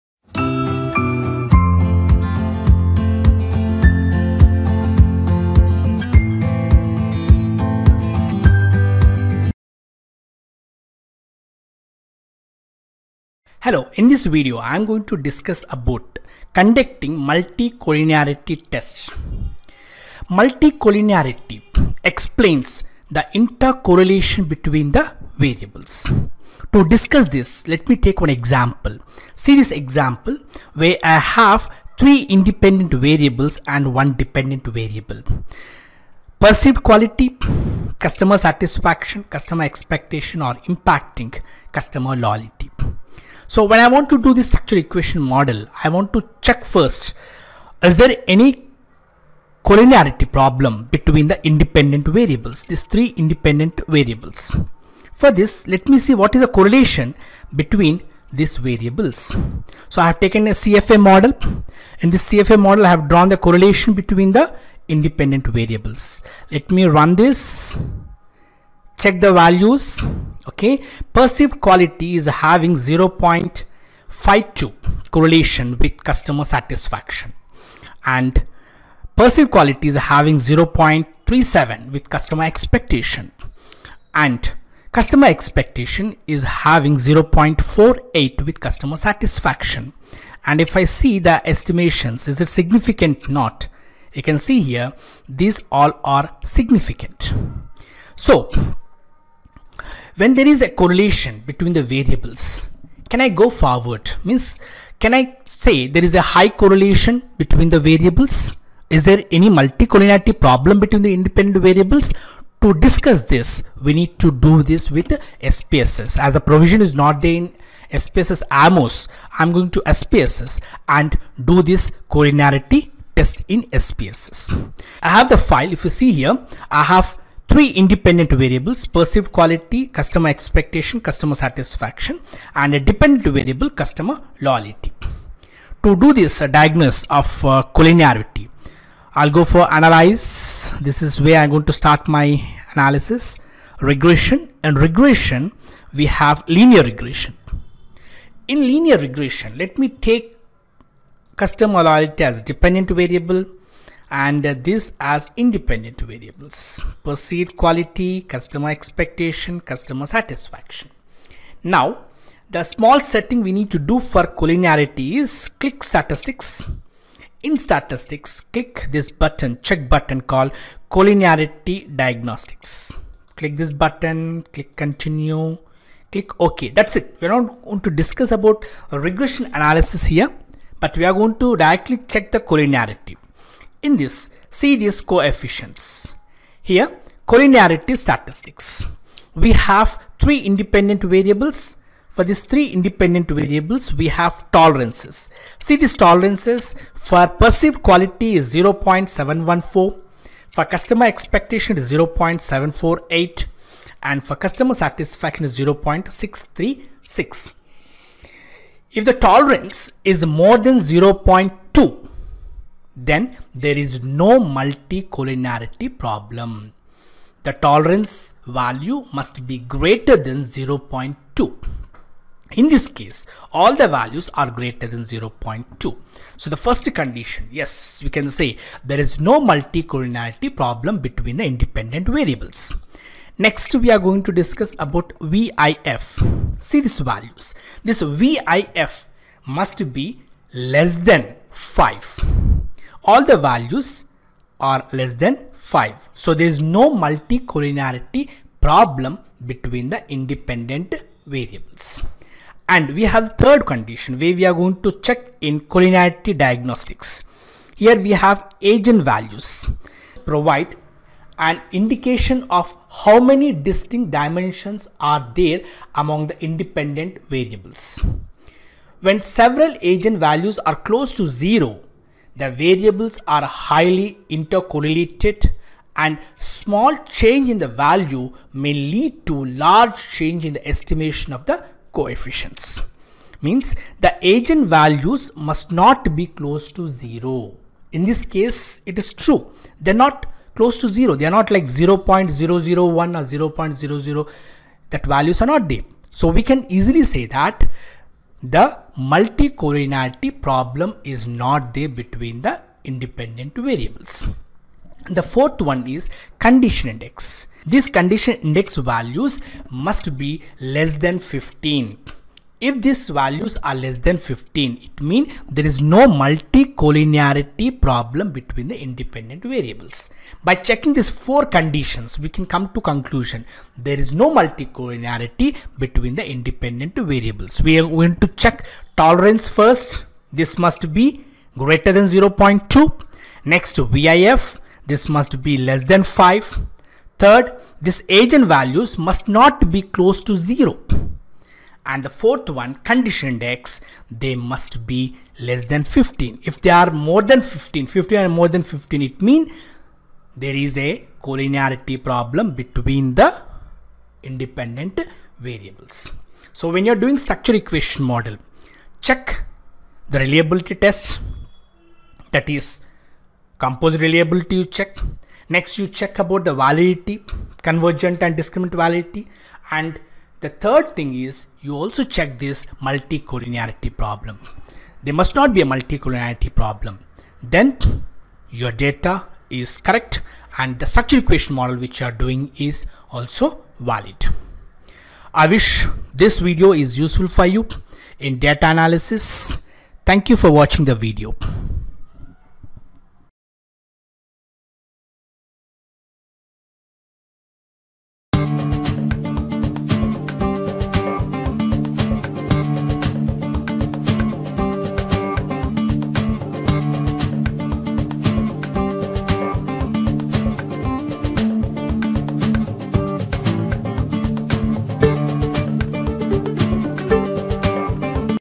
How to do Multi-collinearity test ? # Tolerance test
In this video, I have used SPSS software to test the multicollinearity of the independent variables.
Multicollinearity is the estimation of the intercorrelation between the variables.

There are 4 conditions to check the Independent variable don't have the Multicollinearity
1. The Tolerance must be greater than 0.2

2. The Variance Inflation Factor ( VIF) must be less than 5.0
3. The Eigenvalue must not be close to zero
4. The condition index value must be less than 15.
If the independent variable is having multicollinearity mean small change in the data will lead to a major change in the estimations coefficients.

So, Its always better to check the multicollinearity of the Independent variables.

Please, Like the video and share the video. So, many research people can benefit from the video.